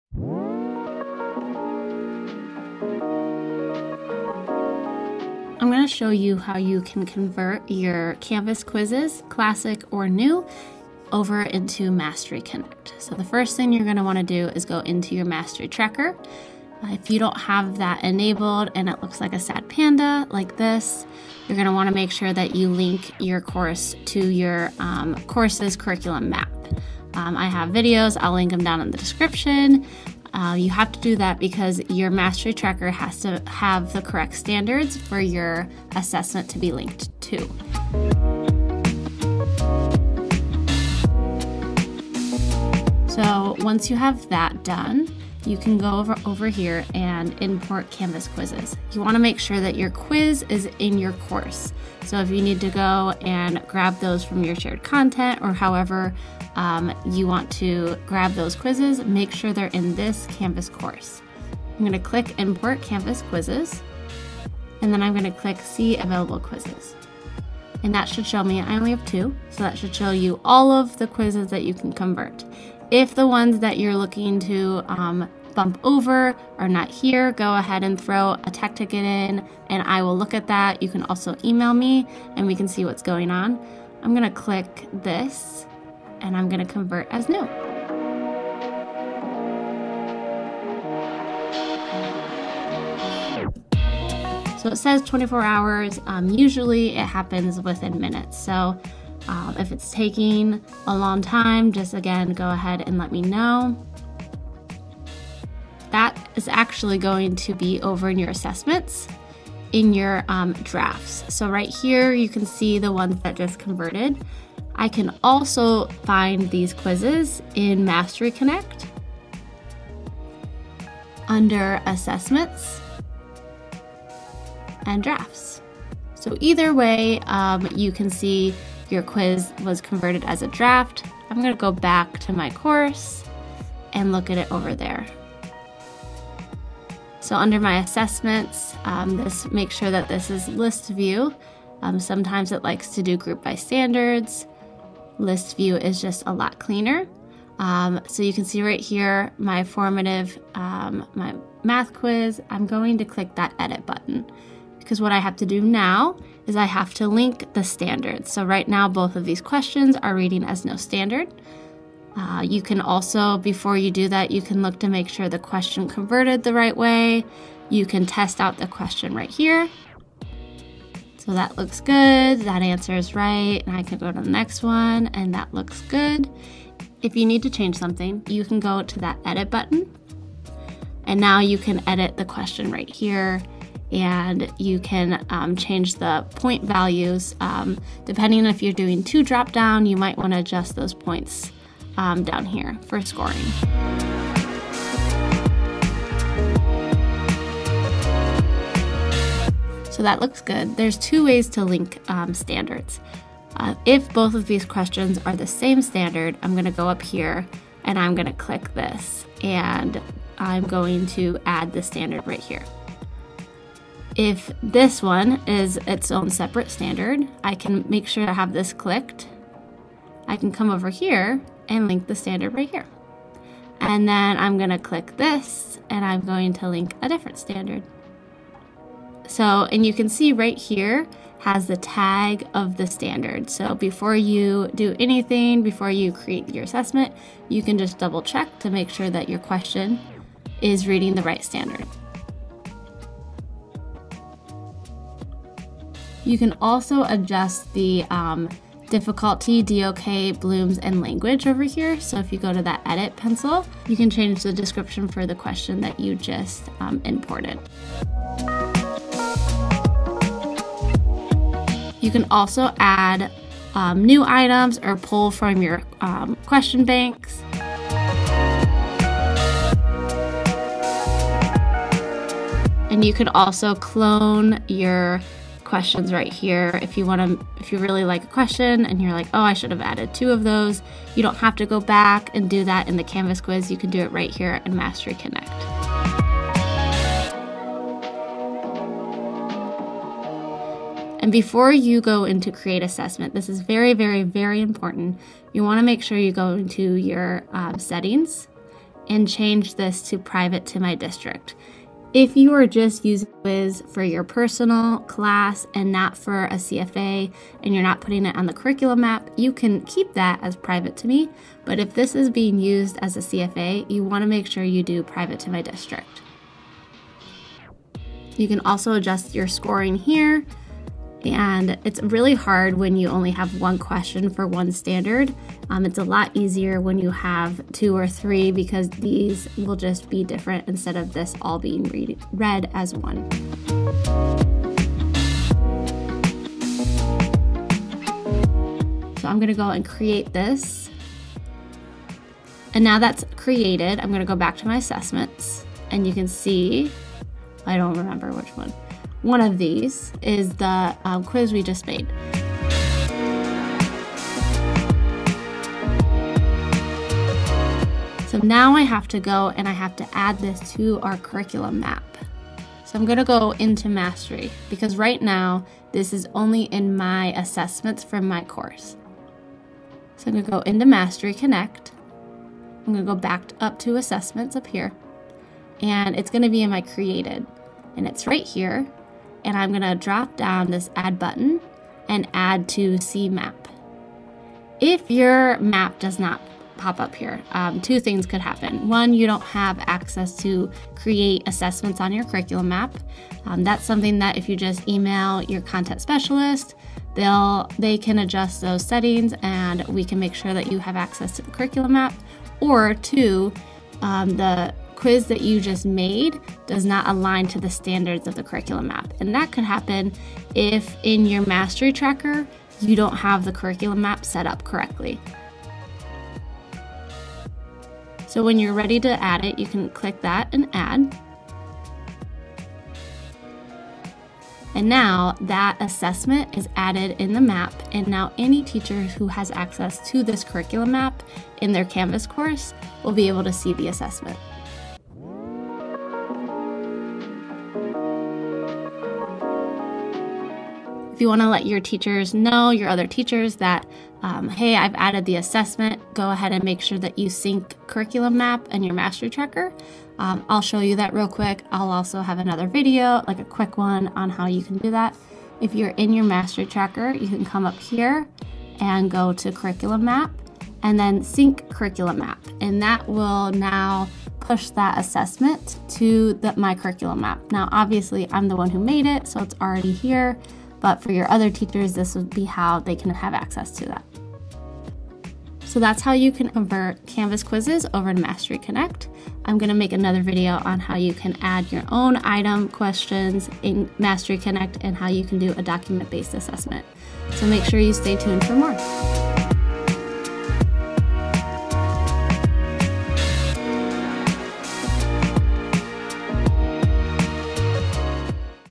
Mastery Connect | Importing your Canvas Quiz
Do you have new or classic canvas quiz you need to import into Mastery Connect? Did you know that you don't have to use Mastery Connect's quizzing system but instead make them on Canvas and just import them! You can also adjust questions, add new ones, and add it right to your course's Curriculum MAP. Check out the chapters below or watch the whole thing.

0:00  Mastery Tracker
0:39 Importing Your Quiz
1:31 Checking the Questions
3:17 Adding Standards
4:18 Adding new Questions
4:29 Cloning Questions
4:50 Quiz Settings
5:38 Creating the Assessment
5:54 Adding to the MAP
7:19 Syncing MAP